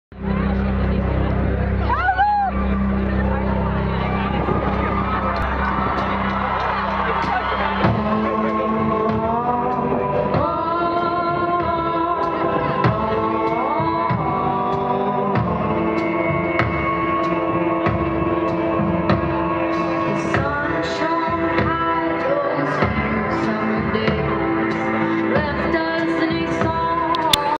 First Aid Kit teaser
Stora scenen, Malmöfestivalen, tisdagen den 19 augusti kl. 21:30